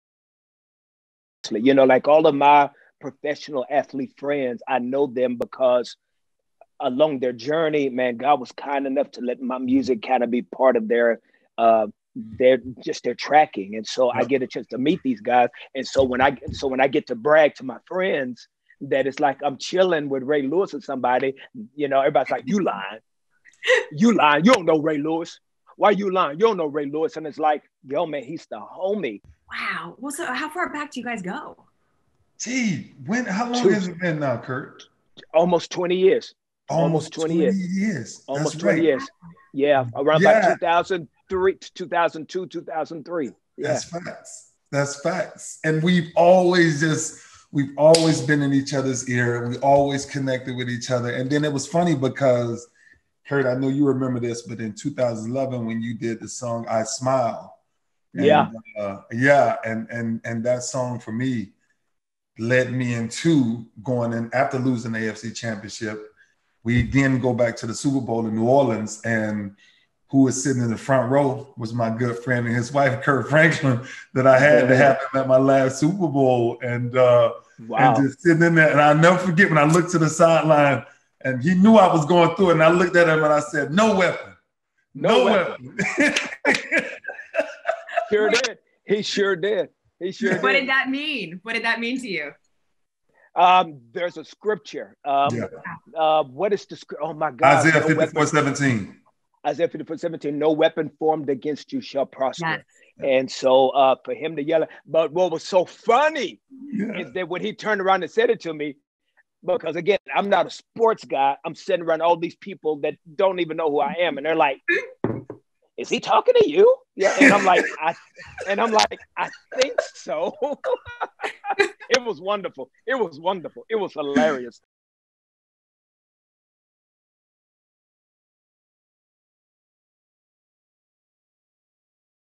"The Artist & The Athlete" Podcast Bits - Ray Lewis and Kirk Franklin on Ray's Super Bowl shoutout
Take a look at some of my favorite excerpts from the conversation I had with Ray Lewis and Kirk Franklin for my new podcast "The Artist and the Athlete", where each week I sit down with one world famous artist and athlete to uncover what makes them tick, and to find some common ground. These celebrities talk no holds bar about their lives and careers - I'm so excited for you to hear what they had to say!

In this bit, Ray Lewis and Kirk Franklin talk being at the Superbowl, and how Franklin's music pulled him through a tough defeat.

See here for more "The Artist & The Athlete" bits!